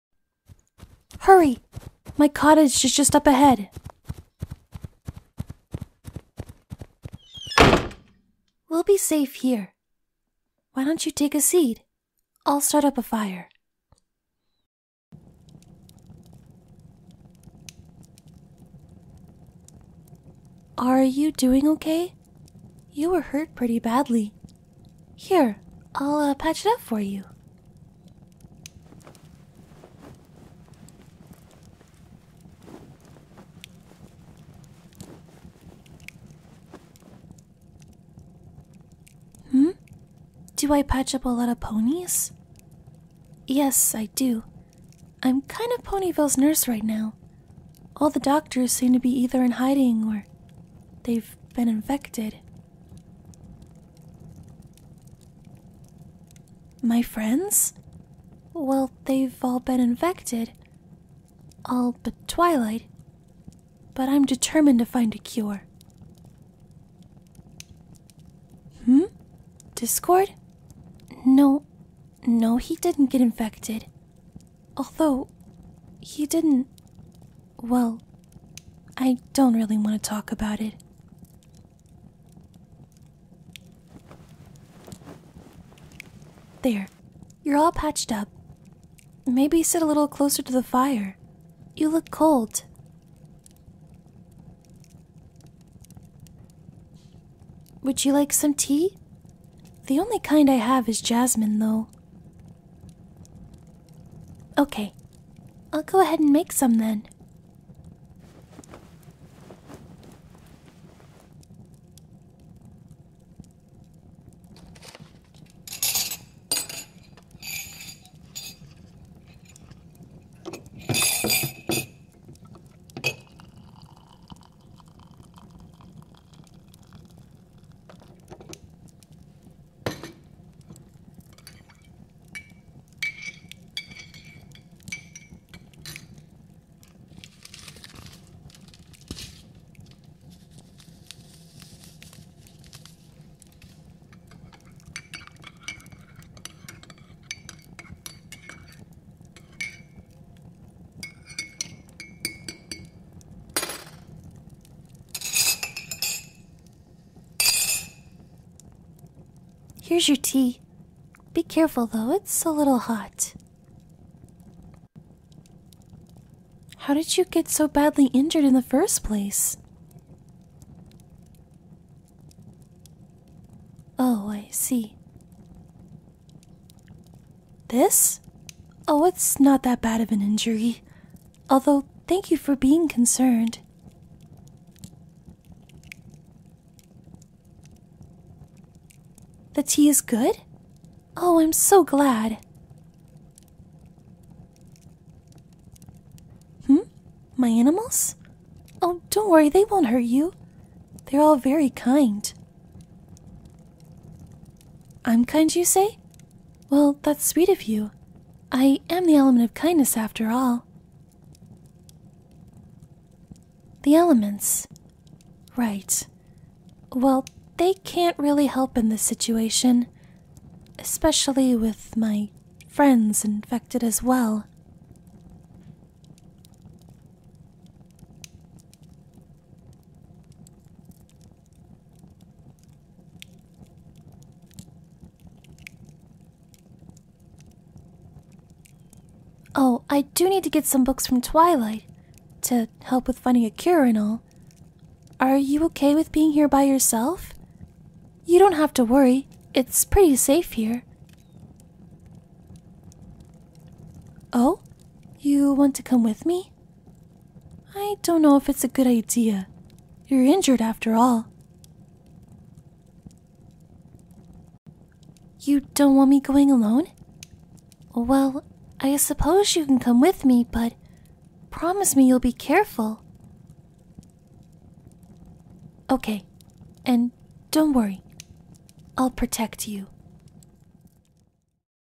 Fluttershy Infection AU (ASMR) Part 1
The first part of my ASMR Infection AU! I hope you enjoy!

Fluttershy- SugarCloud

Art by the amazing Razzyx09